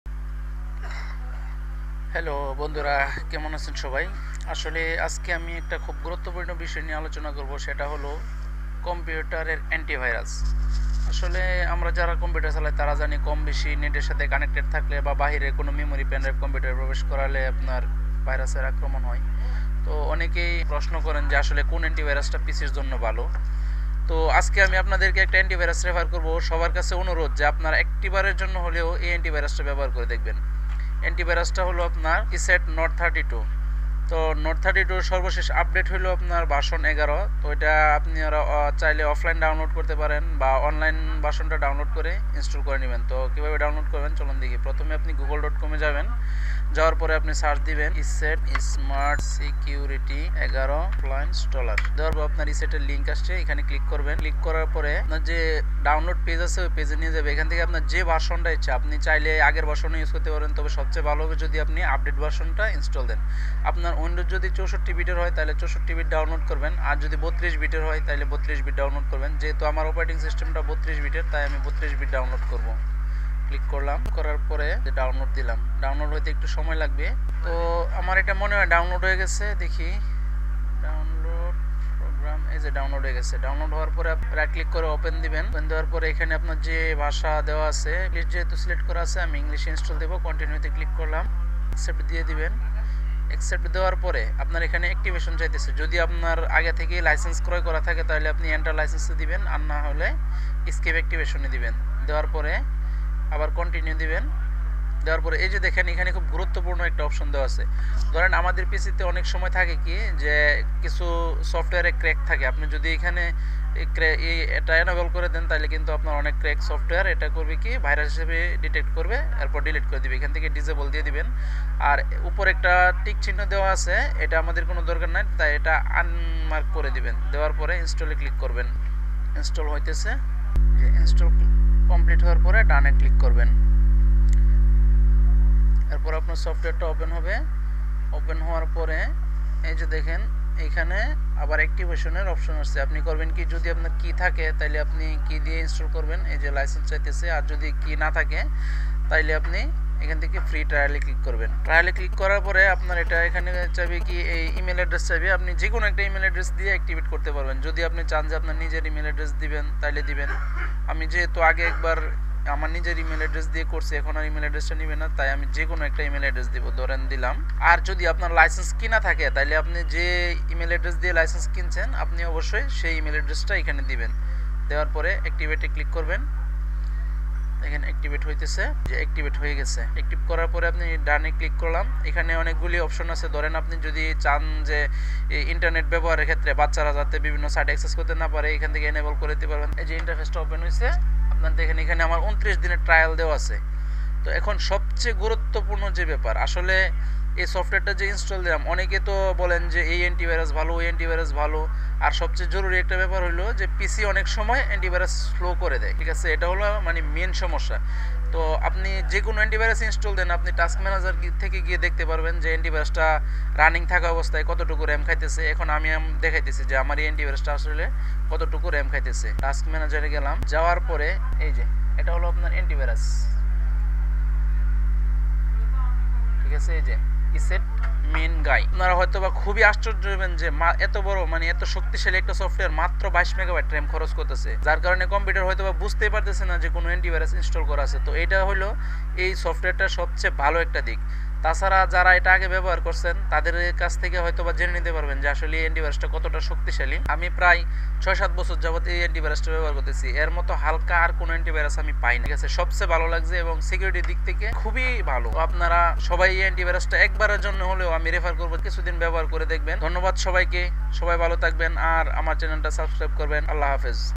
World top anitivirus 2018 Bangla
In this video, I present the top & best antivirus software readily available for one to download, as of 2018.  These antiviruses are completely free for 1 month,  and you do not have to pay a single cent for any of them, unless if you choose to upgrade to the pro versions. This antivirus is compatible with all versions of Windows,
including Windows 7, Windows 8, Windows 8.1, and Windows 10.